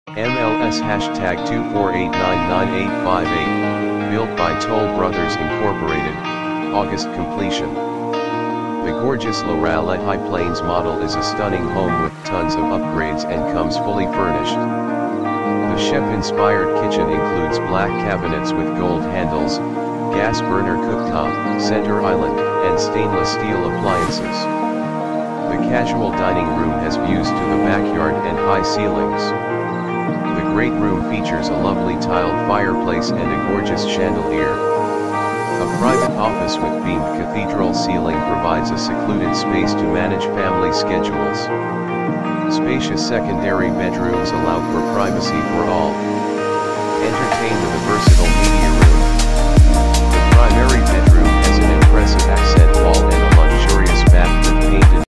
4607 Orchard Creek Lane Manvel TX 77578 Is For Sale Only For $869,813 ($284/Sqft.)
MLS# 24899858 - Built by Toll Brothers, Inc. - August completion! ~ The gorgeous Loralai High Plains model is a stunning home with tons of upgrades and comes fully furnished. The chef-inspired kitchen includes black cabinets with gold handles, gas burner cooktop, center island, and stainless steel appliances. The casual dining room has views to the backyard and high ceilings. The great room features a lovely tiled fireplace and a gorgeous chandelier. A private office with beamed cathedral ceiling provides a secluded space to manage family schedules. Spacious secondary bedrooms allow for privacy for all. Entertain the the versatile media room. The primary bedroom has an impressive accent wall and a luxurious bath with painted tile floor, freestanding tub and dual vanities. This fully furnished, single-story house is the perfect place to call home. The only thing missing is you. shorts